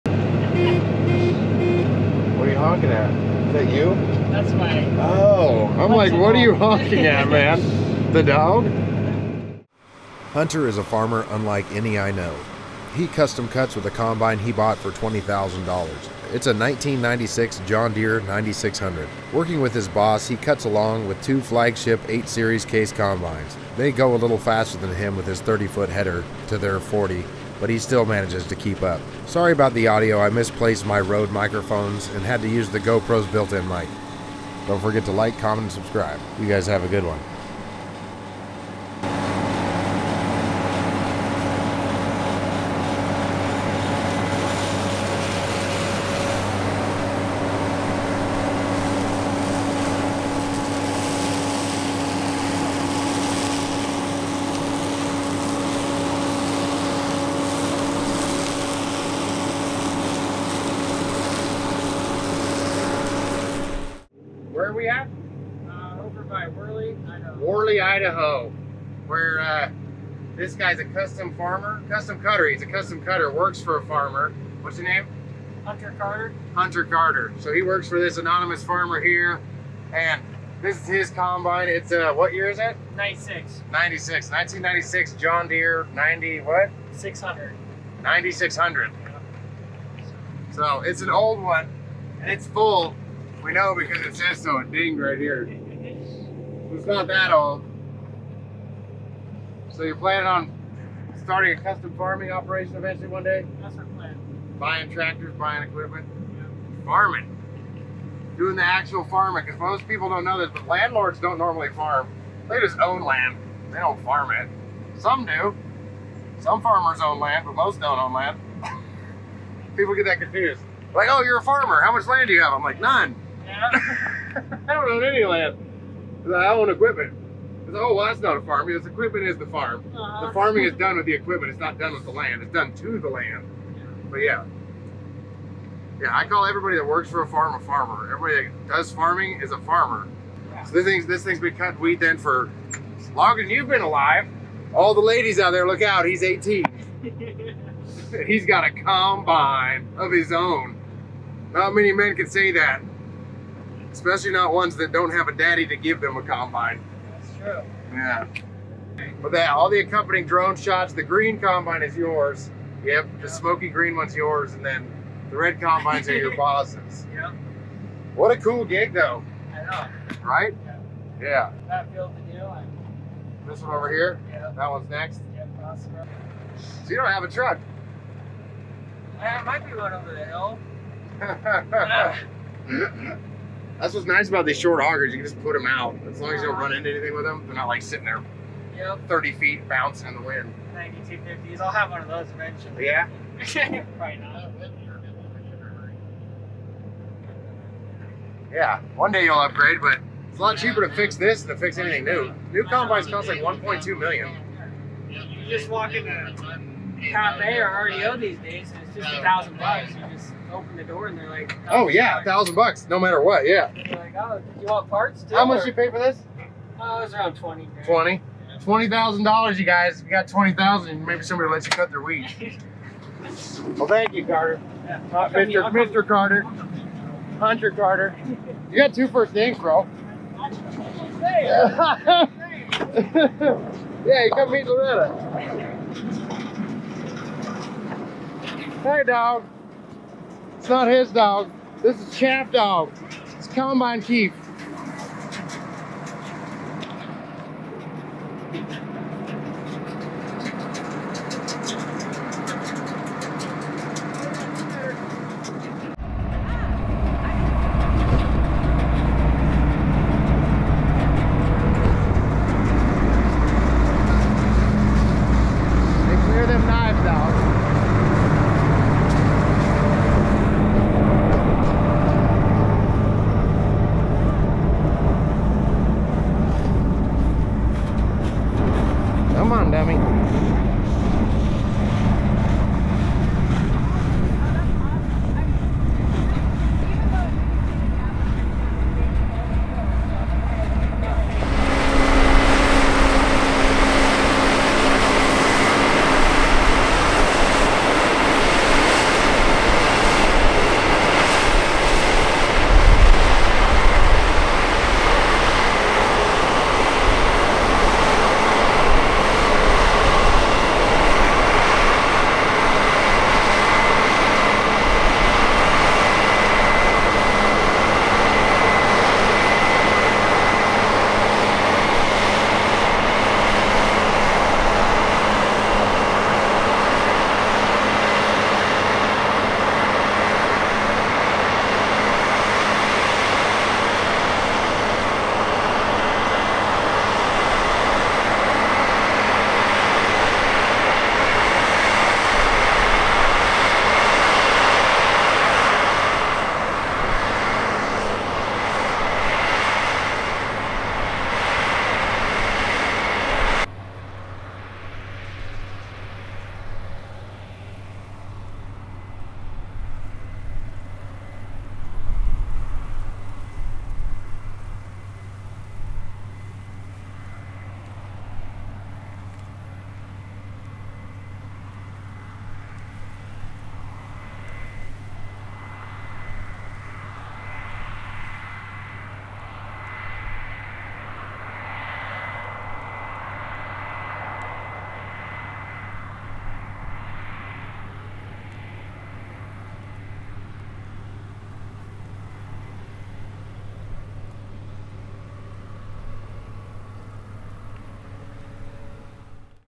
$20,000 Combine Cutting Wheat
Ride along with a custom cutter up in Worley Idaho. Hunter Carter is an amazing dude, he has his own John Deere 9600 combine that he bought. He cuts wheat with it in this video.